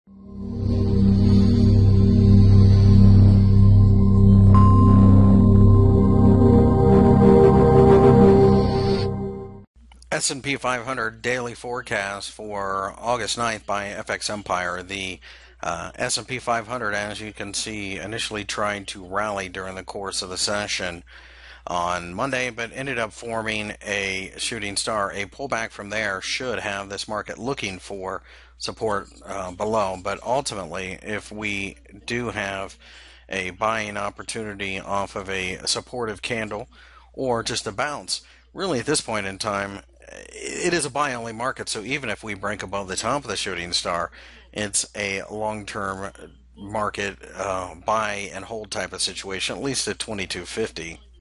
S & P 500 Technical Analysis for August 09 2016 by FXEmpire.com
- August 09 2016 index daily technical analysis for the S & P 500 INDEX. Find more information about Forex and Commodities News Technical Analysis Fundamental Analysis and Brokers on FX Empire